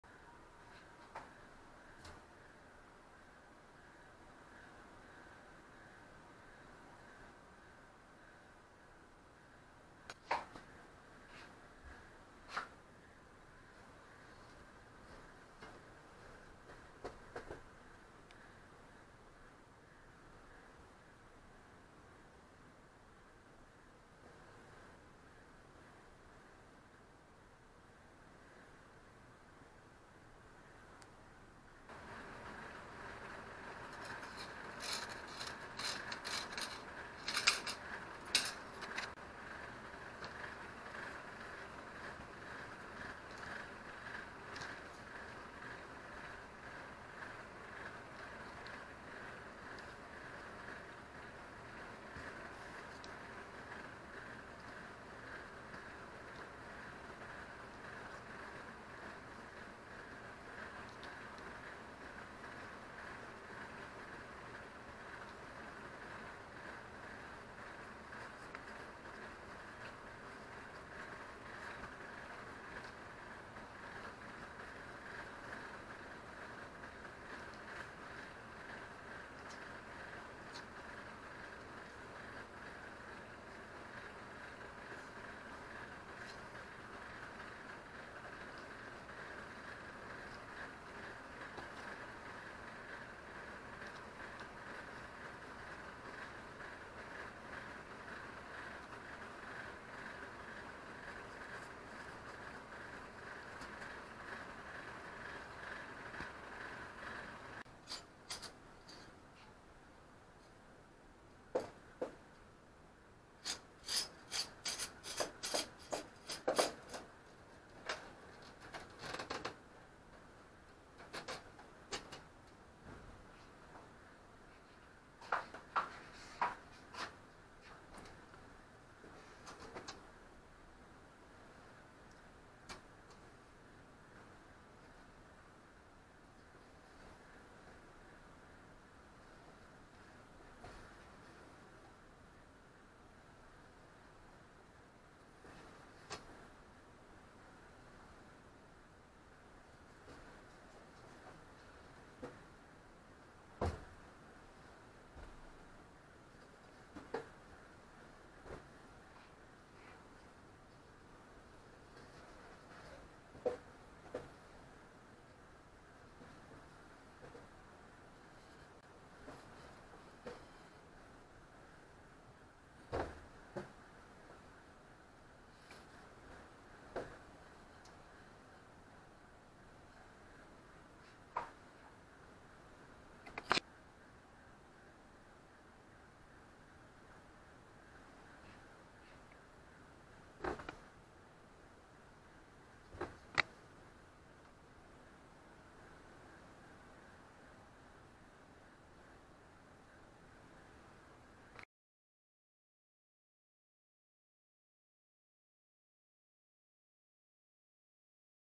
좁은 수건 수납장 편리하게 사용하는법
수건 넣고 꺼낼때마다 불안한 개미 콧구멍만한 수납장, 커튼봉으로 마음 놓고 사용하기